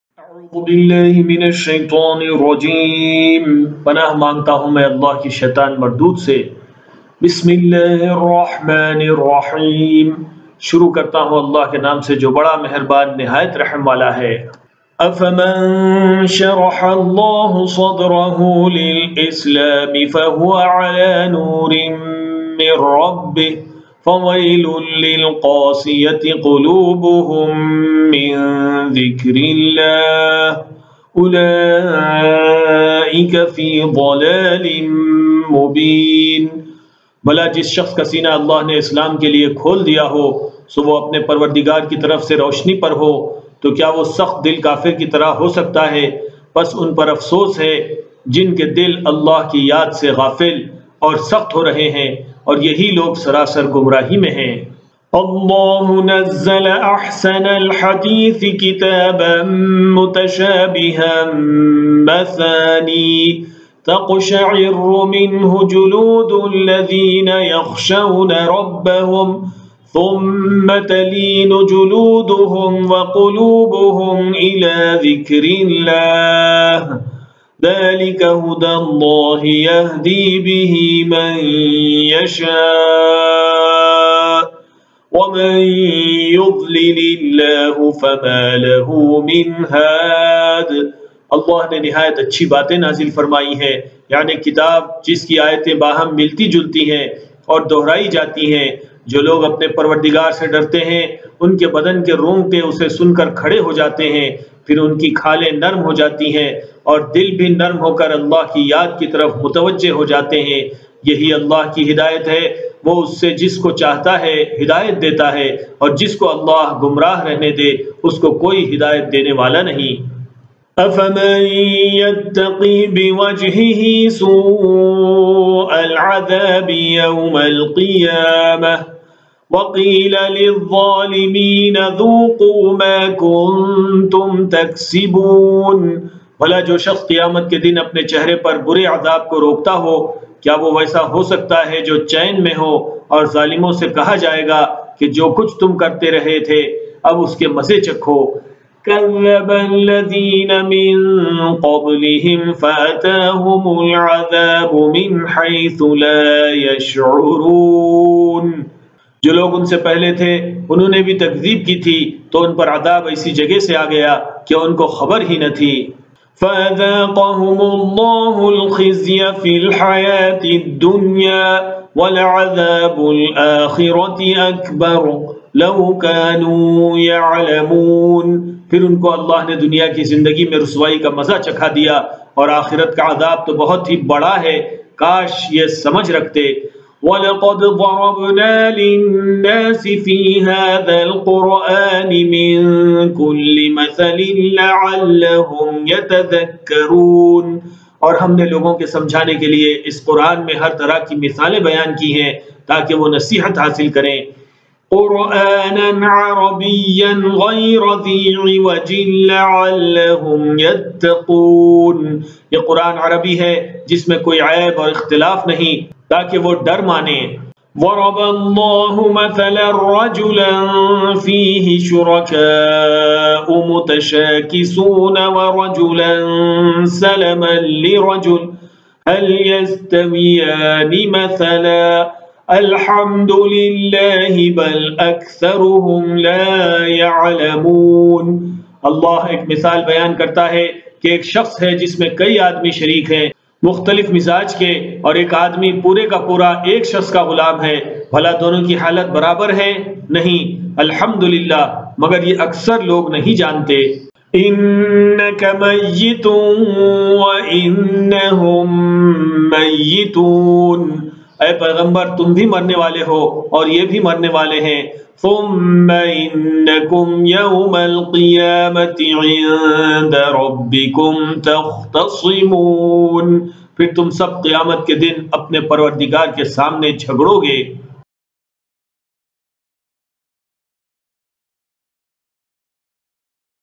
Suratul Zumar Ayat 22-31 with Urdu Translation
Suratul Zumar Ayat 22-31 with Urdu Translation  Tilawat by Qari Muhammed Abdul Mannan, Khateeb Masjid Hibatul Jabbar, Hyderabad, India